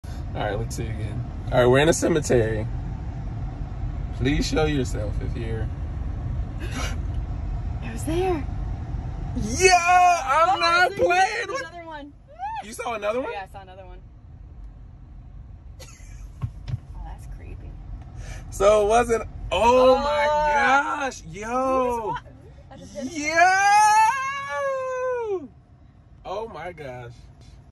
Tesla Spotted Ghosts In The Cemetery tesla elonmusk teslatips 😱
So my friend was showing me her Tesla model 3 and all the cool features so we drove through a grave yard and at first we saw nothing then all of sudden the Tesla starting picking up human movement around the vehicle! So wild! If you have a Tesla or know someone with a Tesla record your experience and tag me I wanna see what you see!